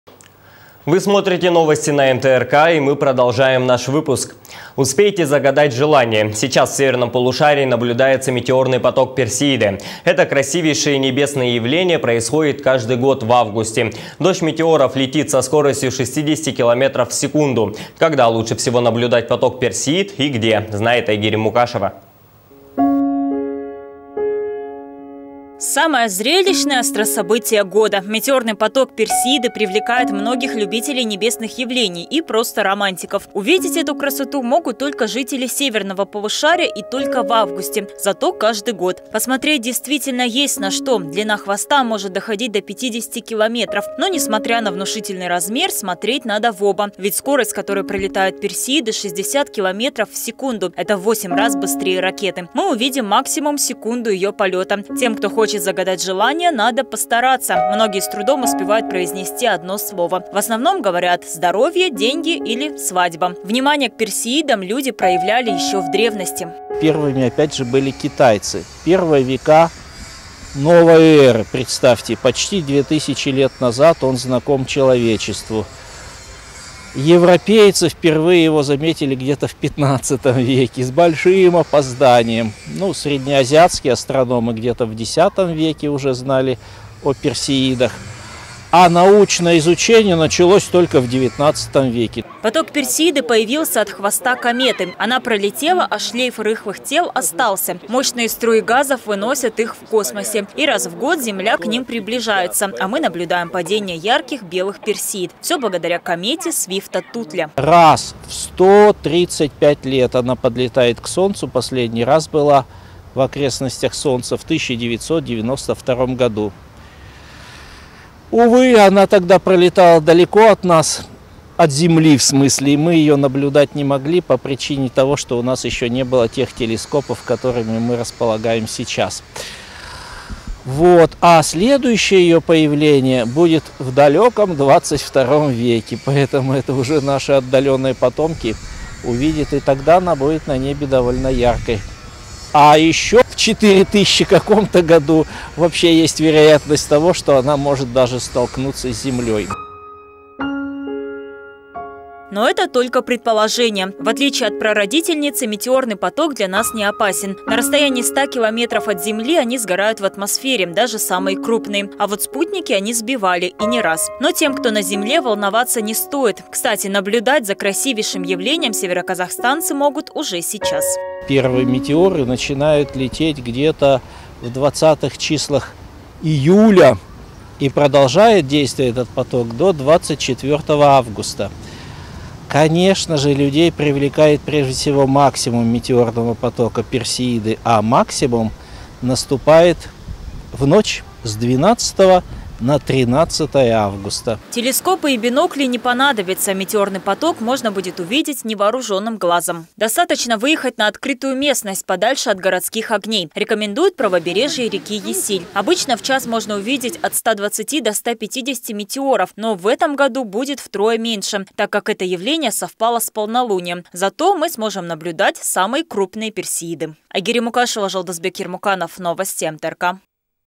Метеорный дождь Персеиды смогут увидеть североказахстанцы
Успейте загать желание! Сейчас в северном полушарии наблюдается метеорный поток ПерсеИды. Это красИвейшее небесное явление происходит каждый год в августе. Дождь метеоров летит со скоростью 60 км/с. Когда лучше всего наблюдать поток персеид и где, знает Айгерим Мукашева.